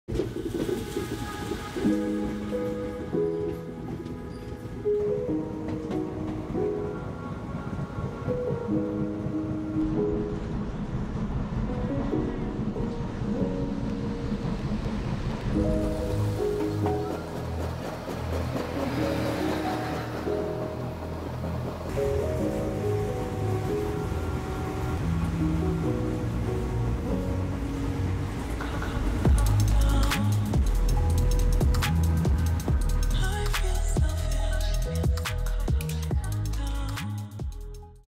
India - Journey on Train
Not every dreams that I dreamt
Had you in it.
Cause life that I lived,
was meant,
for me.
Loving the sound of the train.
Some fast edit of the clips that I took when we were on train from Jaipur - Jaisalmer.